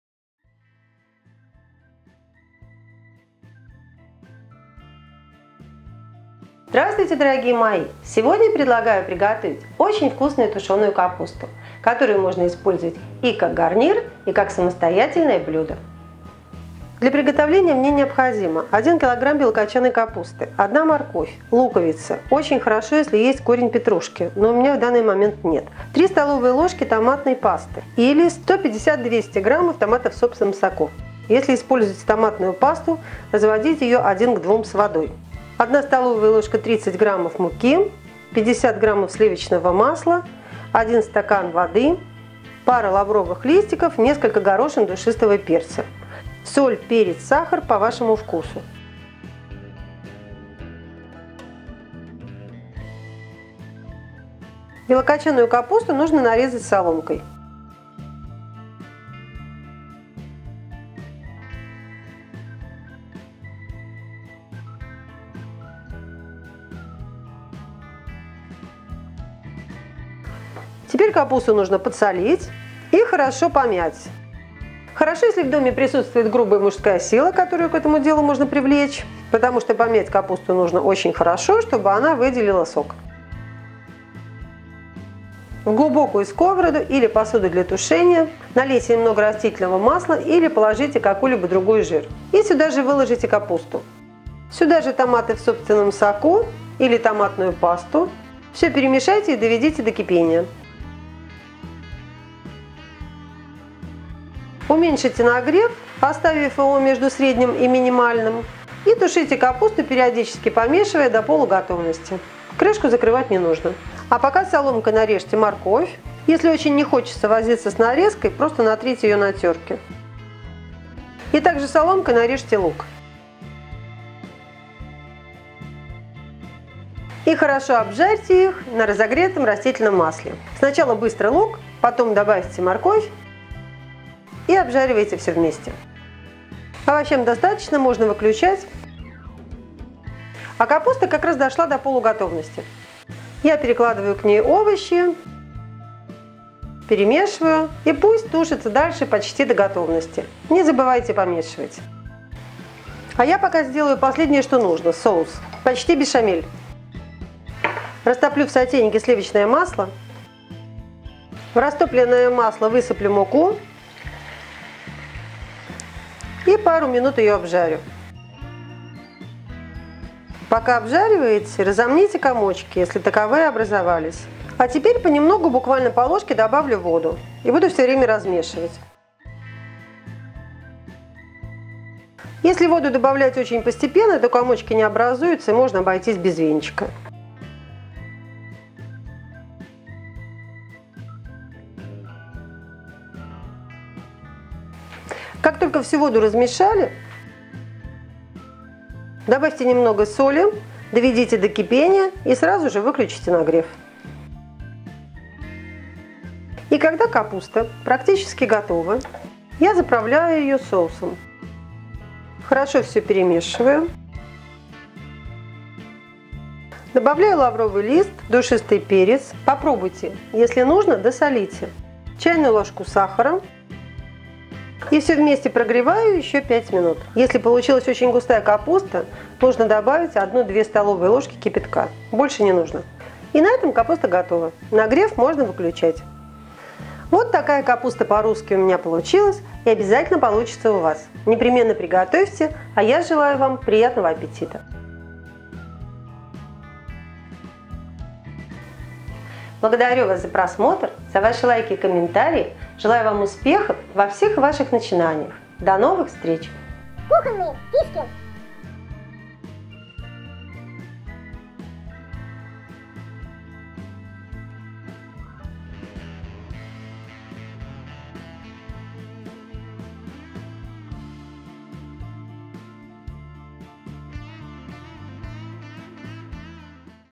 Тушеная КАПУСТА ПО - РУССКИ Самый Вкусный Рецепт Приготовления Капусты
Тушеная капуста по-русски. Рецепт очень вкусной тушеной капусты, которая нравится абсолютно всем. Кто любит капусту с мясом, могут добавить жареную грудинку.